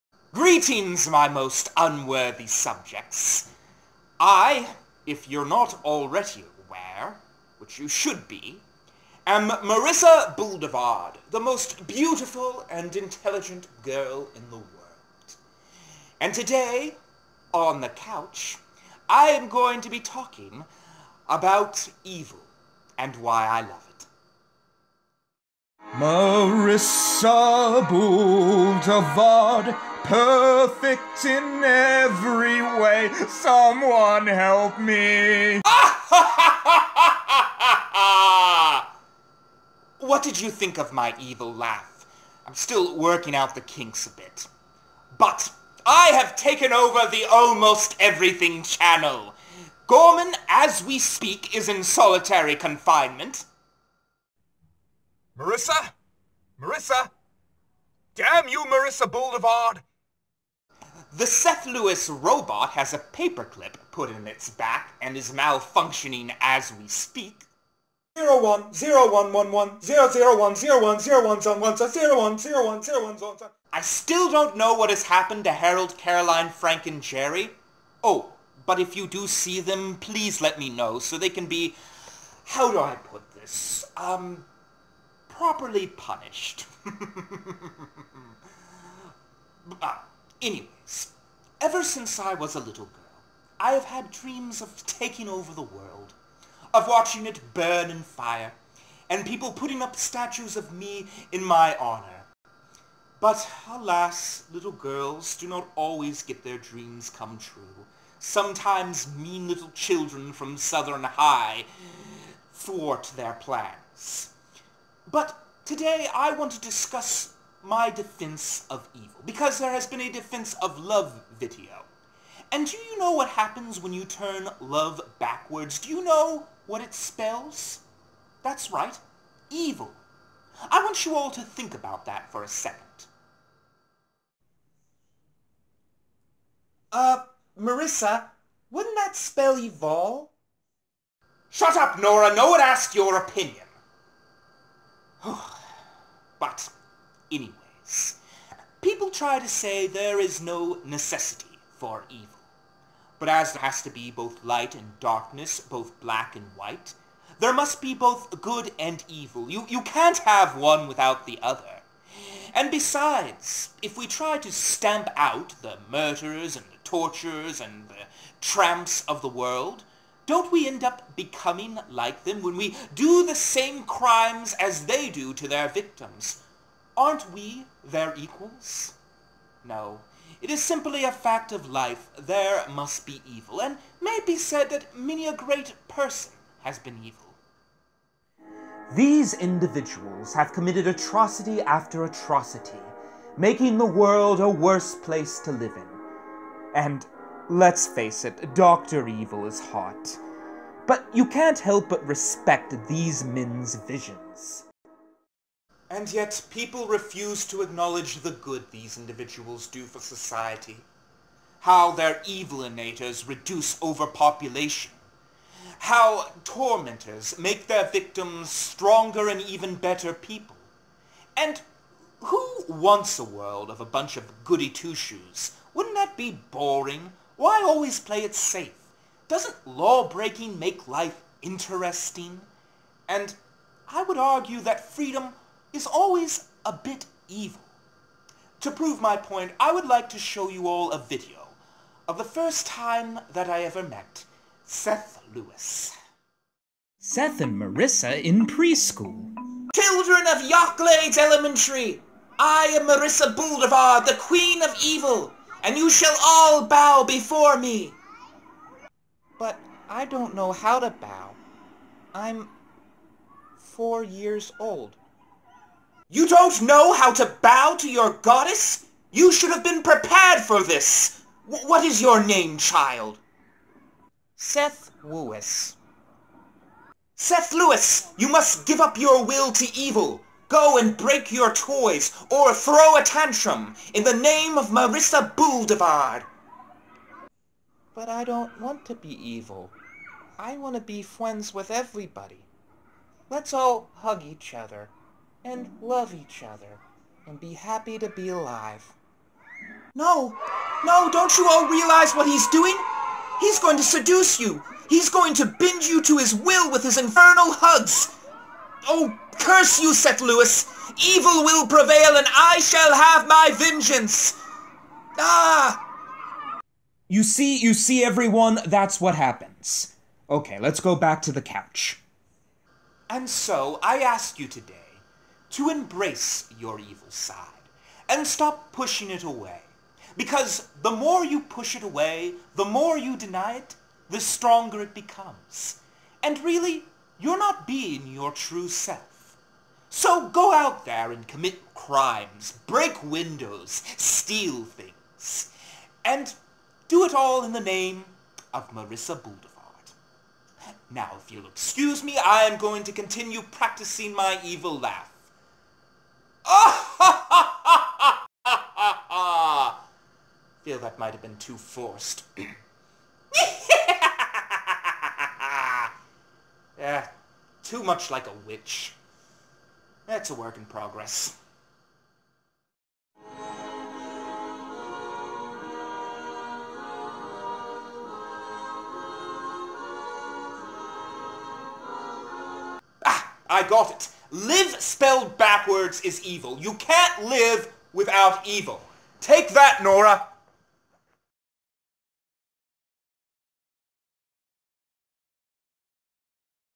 In Defense of Evil
Episode #565. Marissa Boulevard here to discuss evil...and why I love it. As you know, I have taken over the "Almost Everything," channel and soon plan to take over the world! Enjoy the world why you can you fools. HA! HA! HA! HA!